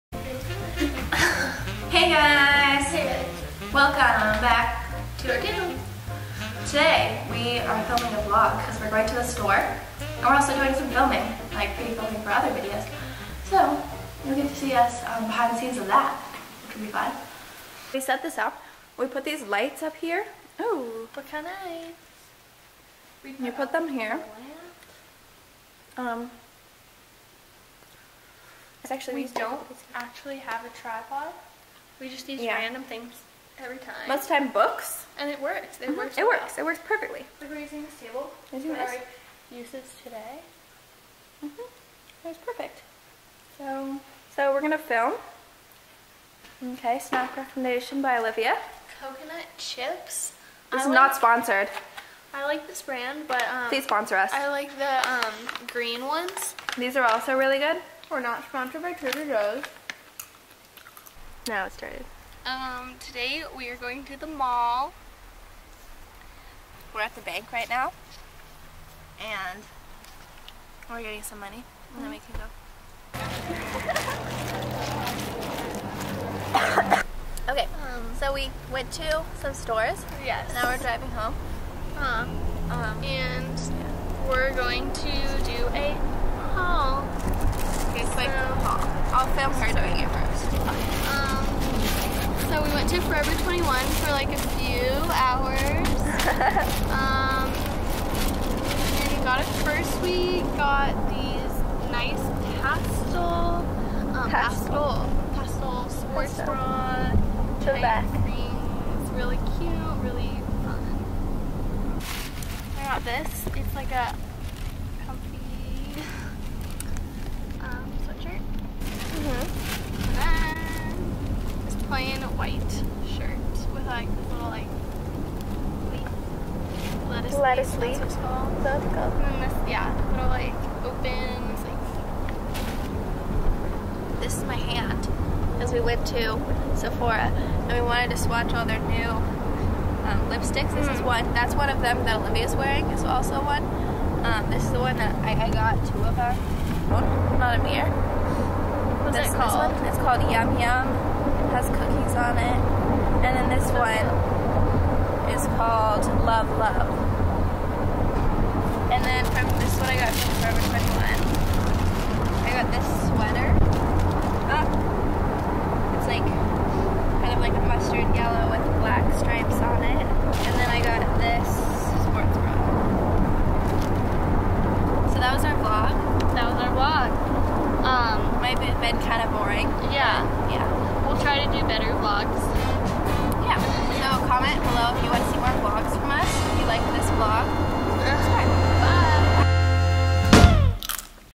Behind the scenes & mini vlog! GabbyandOlivia
Hey guys!

Here is a short vlog showing some behind the scenes of us filming, and what we got when we went shopping!